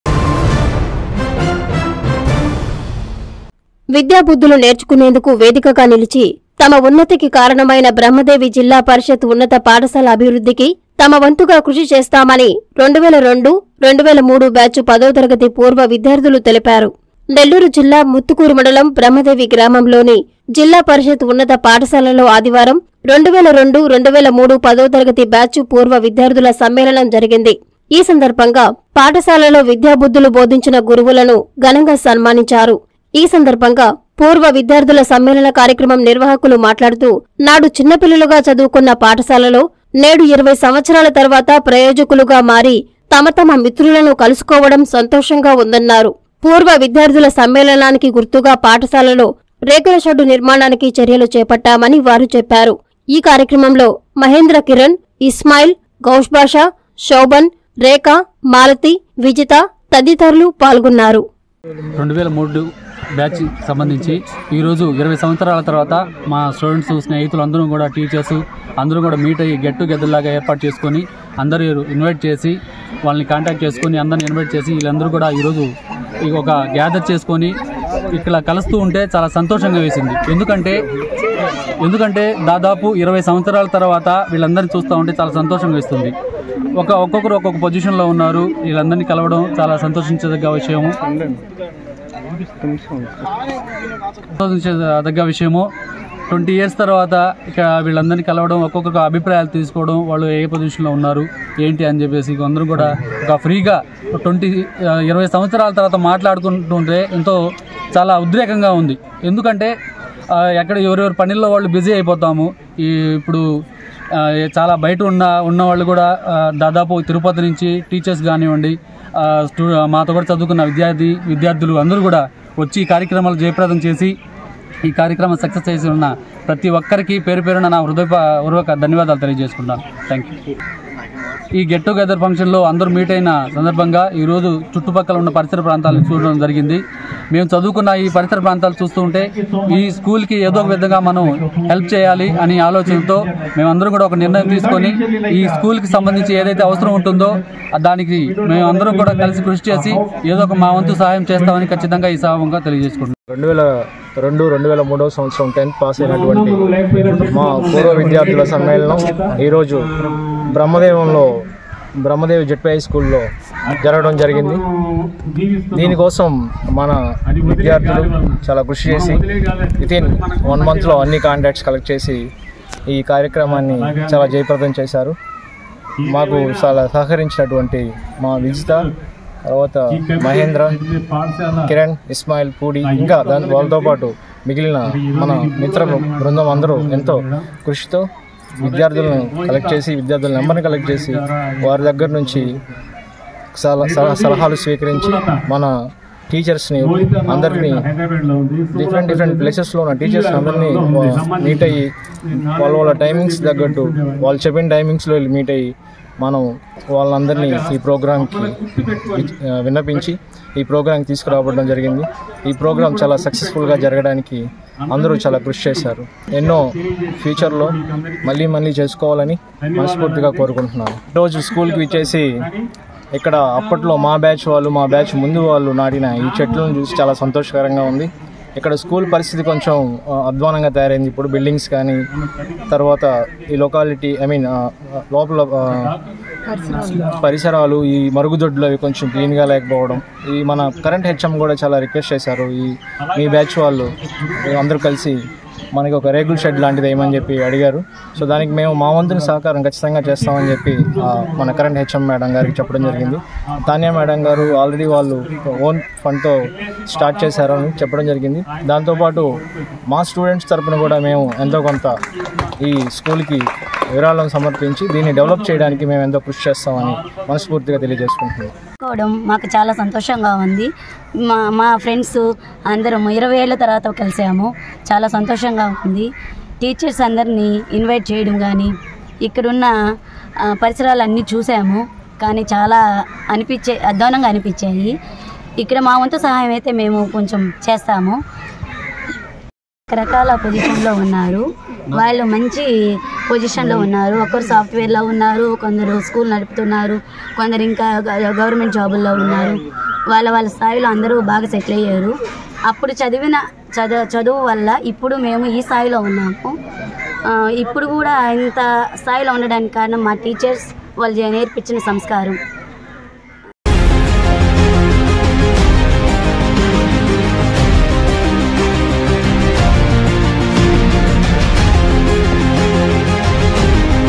పాఠశాల అభివృద్ధికి తోడ్పడుతాం | Alumni Meet at Muthukuru | Old Students Honor Teachers
- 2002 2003 బ్యాచ్ పూర్వ విద్యార్థుల సమ్మేళనం...
- గురువులను ఘనంగా సన్మానించిన పూర్వ విద్యార్థులు...